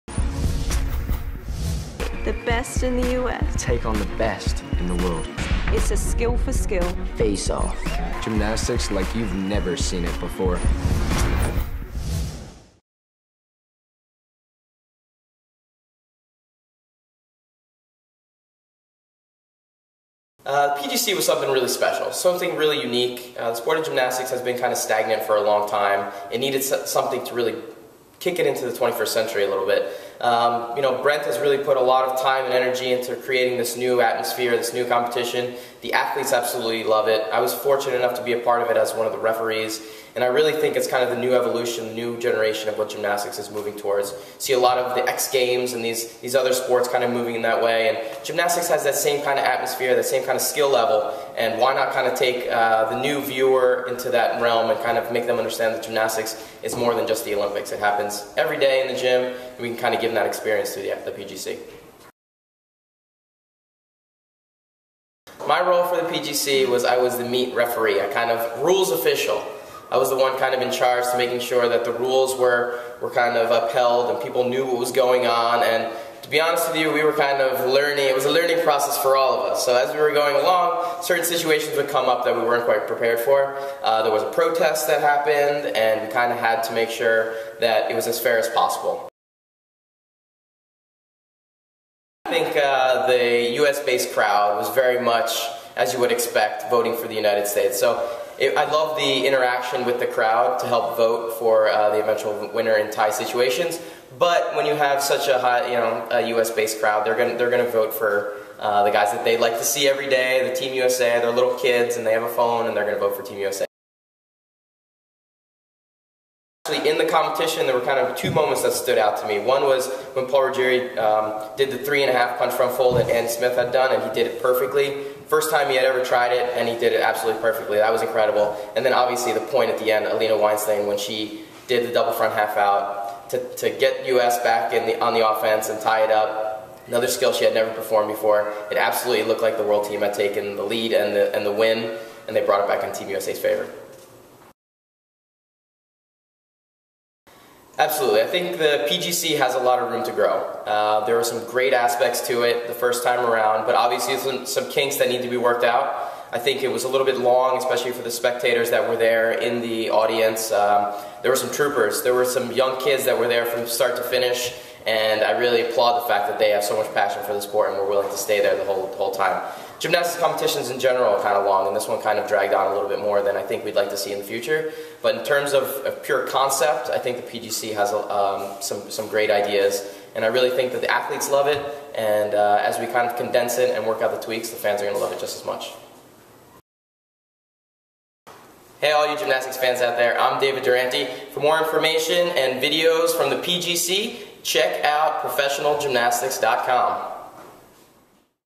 PGC SESSIONS: Dave Durante
Dave Durante talks Professional Gymnastics Challenge! and Twitter @ProGymChallenge